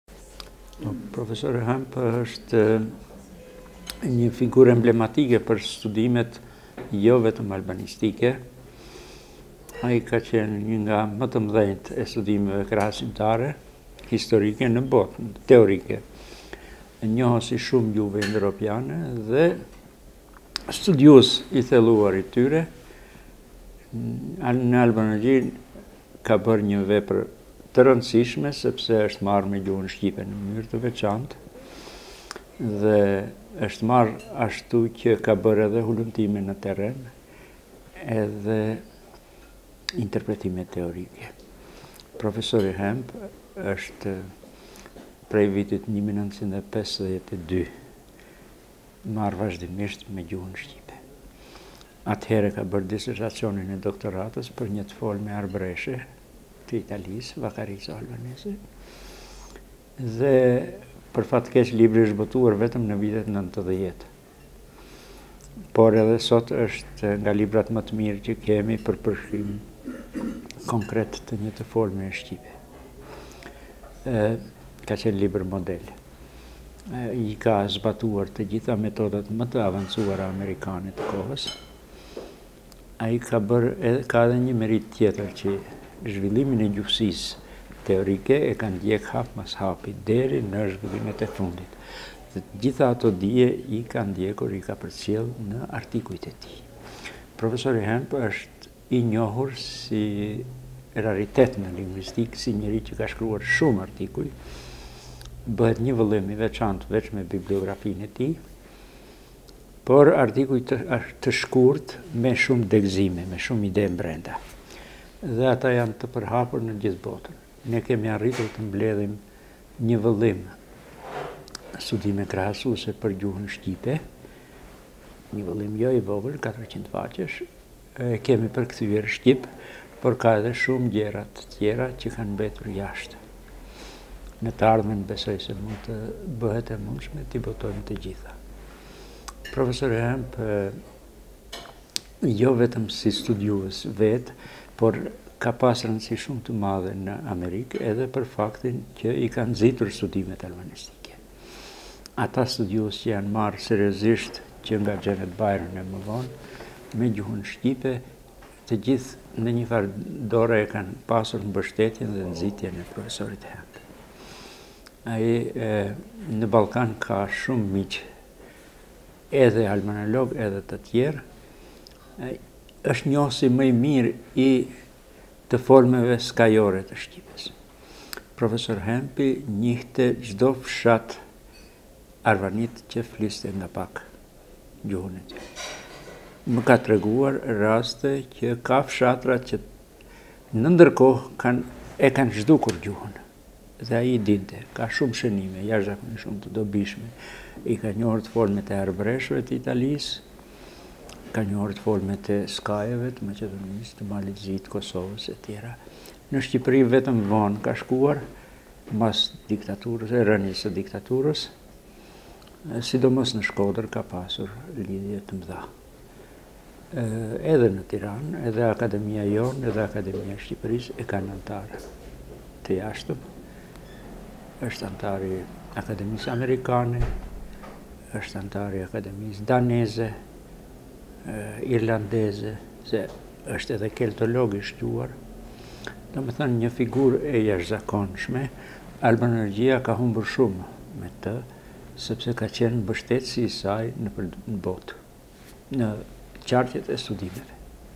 REXHEP ISMAJLI PËR PROF. ERIC HAMP
Akademik Rexhep Ismajli kujton profesorin e gjuhësisë Eric Hamp.